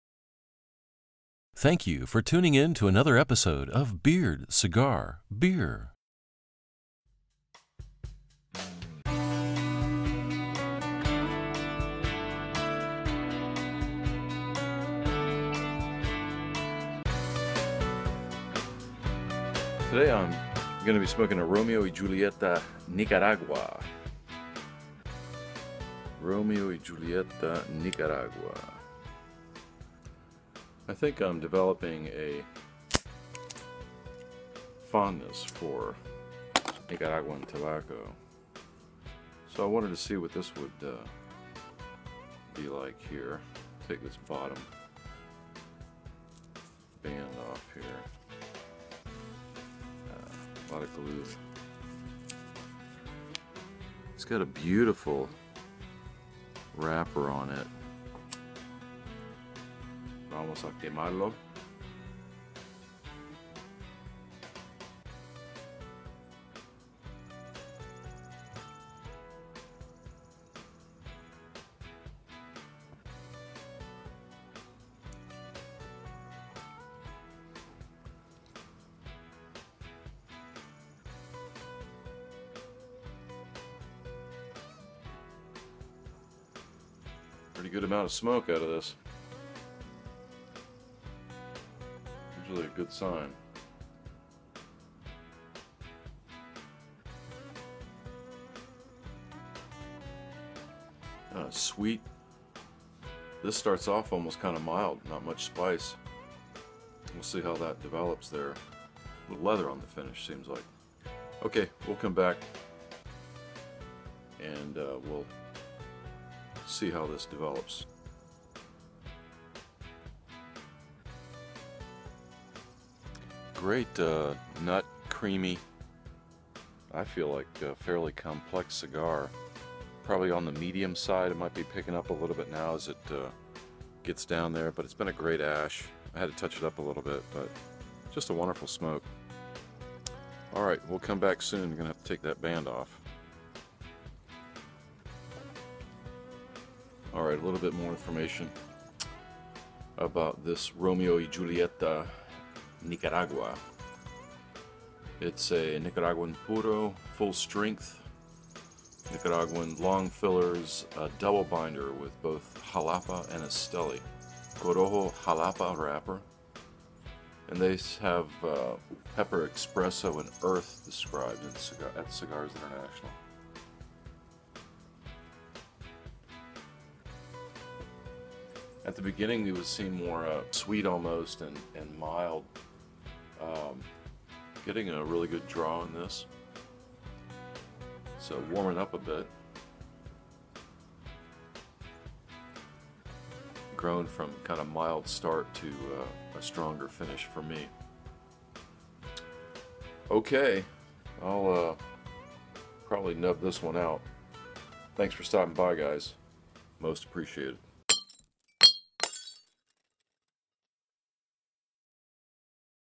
Episode - 87 [Romeo y Julieta - Nicaragua]
Strength: Full
Wrapper: Nicaraguan
Binder: Nicaraguan Jalapa and Esteli
Origin: Nicaragua

-Cheers!